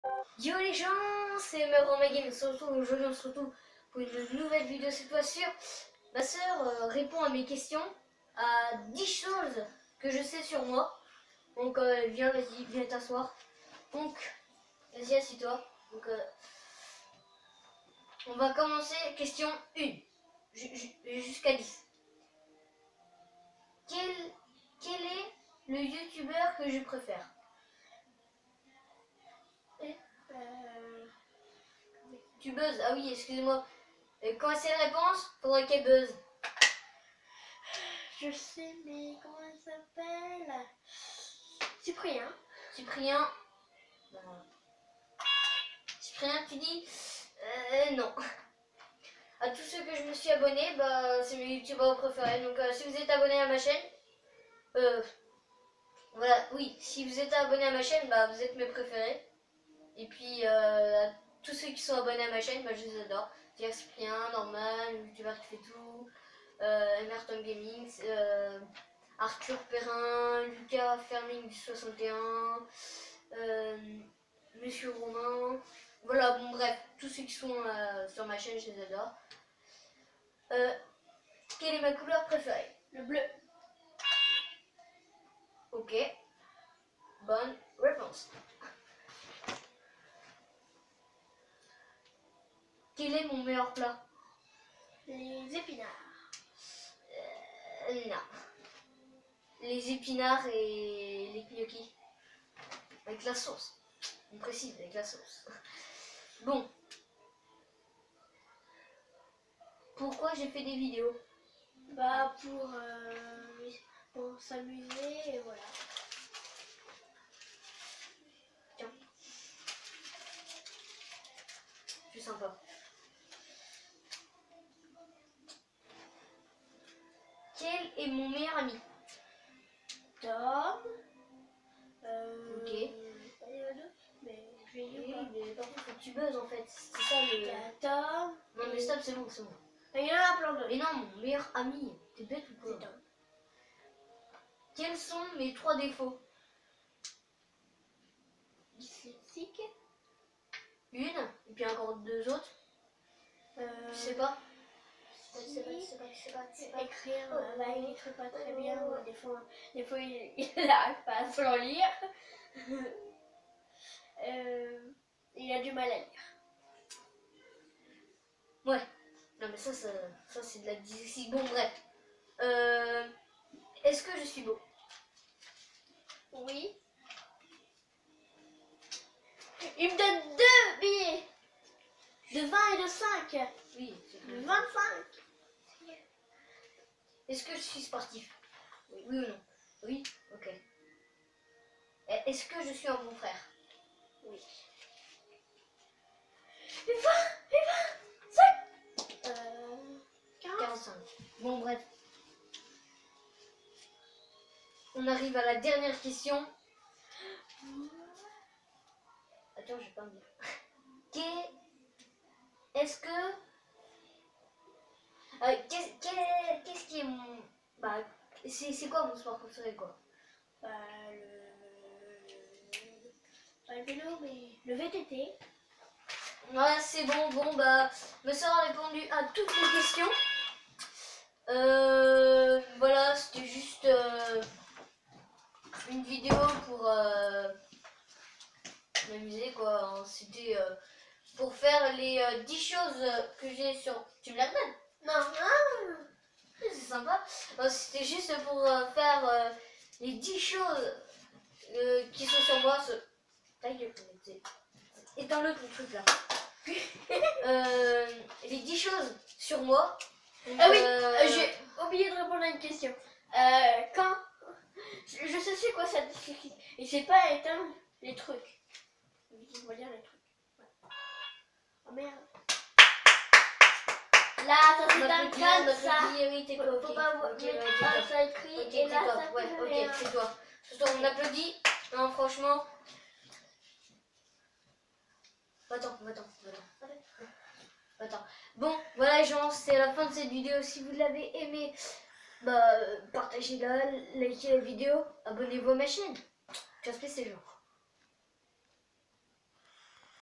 Ma sœur répond a mes questions (10 chose sur moi)
Je ai fait une vidéo sur les 10 chose sur moi